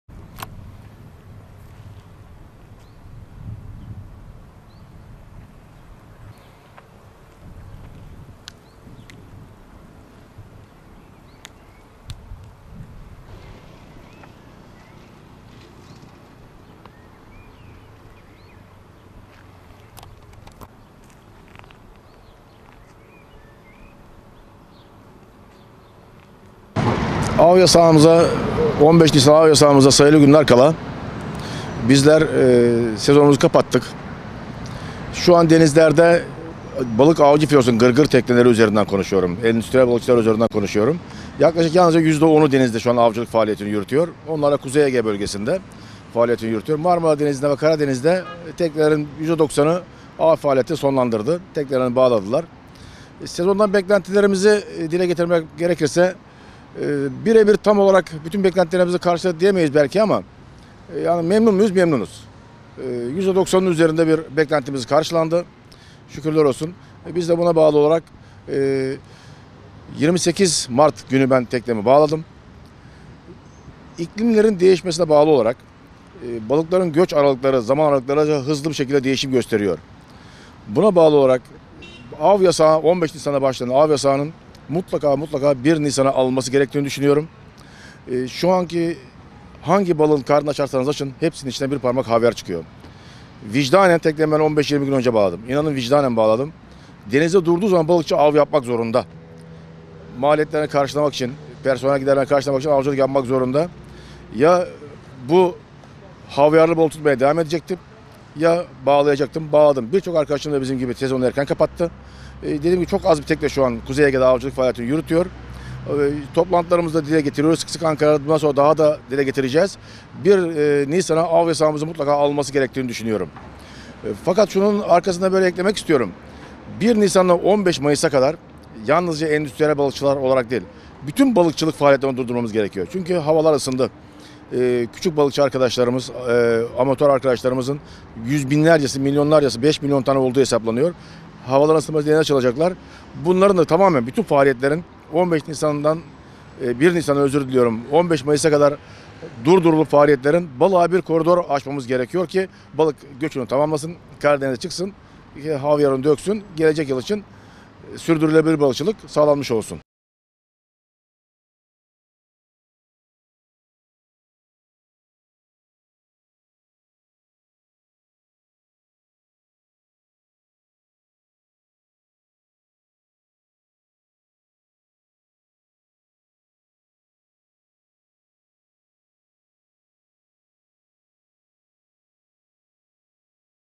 ‘İklim değişikliği nedeniyle denizlerde av yasağı geriye çekilmeli’
Deniz Ürünleri Avcıları Üreticileri Merkez Birliği (DEM-BİR) Çanakkale-Tekirdağ Bölge Birliği Başkanı Naci Karabiber, iklim değişikliği nedeniyle sürdürebilir balıkçılık için av yasağının geriye çekilmesi gerektiğini belirtip, "İklimlerin değişmesine bağlı olarak balıkların göç aralıkları hızlı bir şekilde değişim gösteriyor. Bu nedenle 15 Nisan'da başlayan av yasağının mutlaka 1 Nisan'a alınması gerektiğini düşünüyorum. Şu an hangi balığın karnını açarsanız açın hepsinin içinde bir parmak havyar çıkıyor" dedi.

Çanakkale Boğazı'nda ve uluslararası sularda gırgır, trol ve çevirme ağlarına 15 Nisan itibarıyla av yasağı başlıyor. Av sezonu hakkında değerlendirmelerde bulunan DEM-BİR Çanakkale-Tekirdağ Bölge Birliği Başkanı Naci Karabiber, iklim değişikliği nedeniyle sürdürebilir balıkçılık için av yasağının 1 Nisan tarihine alınması gerektiğini söyledi. Çok sayıda balıkçının av sezonunu kapattığını söyleyen Naci Karabiber, "Şu an denizlerde, endüstriyel balıkçılar üzerinden konuşuyorum, yalnızca yüzde 10'u denizde avcılık faaliyetini yürütüyor. Onlar da Kuzey Ege Bölgesi'nde faaliyetini yürütüyor. Marmara Denizi'nde ve Karadeniz'de teknelerin yüzde 90'ı ağ faaliyeti sonlandırdı, teknelerini bağladı. 'Sezonda tam olarak bütün beklentilerimizi karşıladık' diyemeyiz belki ama memnunuz. Yüzde 90'ın üzerinde bir beklentimiz karşılandı" ifadelerini kullandı.

1 NİSAN'A, 15 MAYIS'A KADAR BÜTÜN BALIKÇILIK FAALİYETLERİNİ DURDURMAMIZ LAZIM'

Kendi teknesini 28 Mart'ta bağladığını söyleyen Karabiber, "İklimlerin değişmesine bağlı olarak balıkların göç zamanları hızlı bir şekilde değişim gösteriyor. Buna bağlı olarak 15 Nisan'a başlayan av yasağının mutlaka 1 Nisan'a alınması gerektiğini düşünüyorum. Şu an hangi balığın karnını açarsanız açın hepsinin içine bir parmak havyar çıkıyor. Vicdanen teknemi 15-20 gün önce bağladım. Balıkçı, denizde durduğu zaman av yapmak zorunda. Maliyetlerini ve personel giderlerini karşılamak için avcılık yapmak zorunda. Ya bu havyarlı balığı tutmaya devam edecektim ya bağlayacaktım, bağladım. Birçok arkadaşım da bizim gibi sezonu erken kapattı. Çok az bir tekne şu an Kuzey Ege'de avcılık faaliyetini yürütüyor. Toplantılarımızda da dile getiriyoruz. Bundan sonra da sık sık dile getireceğiz. Av yasağının mutlaka 1 Nisan tarihine alınması gerektiğini düşünüyorum. 1 Nisan ile 15 Mayıs'a kadar yalnızca endüstriyel balıkçılar olarak değil, bütün balıkçılık faaliyetlerini durdurmamız gerekiyor. Çünkü havalar ısındı. Küçük balıkçı arkadaşlarımız, amatör arkadaşlarımızın yüz binlercesi, milyonlarcası, havaların ısınması nedeniyle denize açılacaklar. Bütün faaliyetlerin 1 Nisan'a, 15 Mayıs'a kadar durdurulup balığa bir koridor açılması gerekiyor ki balık göçünü tamamlasın, Karadeniz'e çıksın, havyarını döksün, gelecek yıl için sürdürülebilir balıkçılık sağlanmış olsun" diye konuştu. (DHA)

Haber-Kamera: Nazif Cemhan ŞEN/ ÇANAKKALE, (DHA)